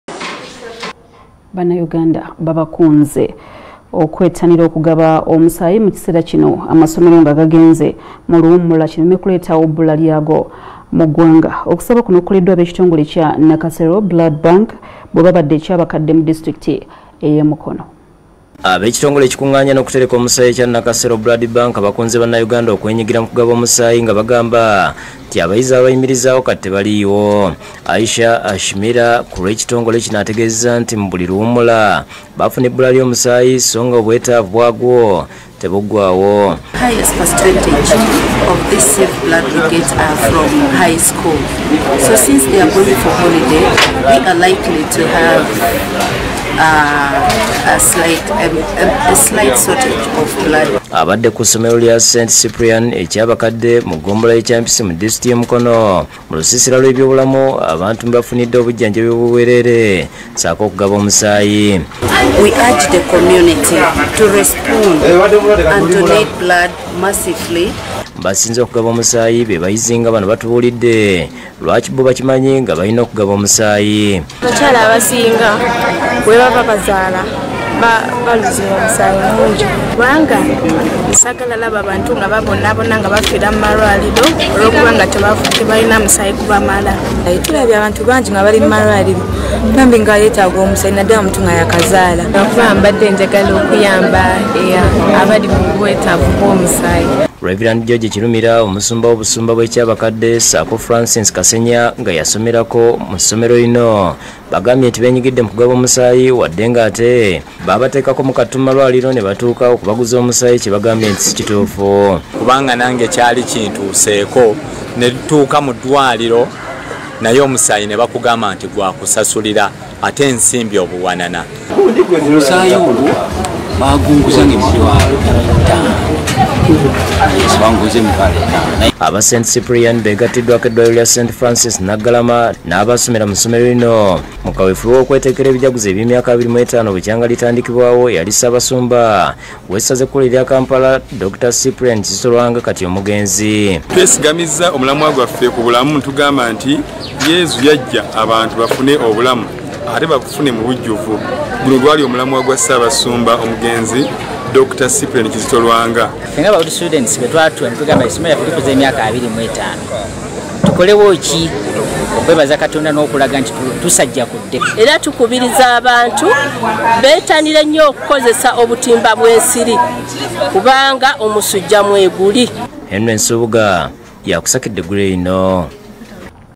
OKUMALAWO EBBULA LY'OMUSAAYI MU LUWUMMULA, ABA NAKASERO BLOOD BANKA BAWABUDDE, BASABYE BANNAYUGANDA
OKUMALAWO EBBULA LY'OMUSAAYI MU LUWUMMULA, ABA NAKASERO BLOOD BANKA BAWABUDDE, BASABYE BANNAYUGANDA OKUGUGABA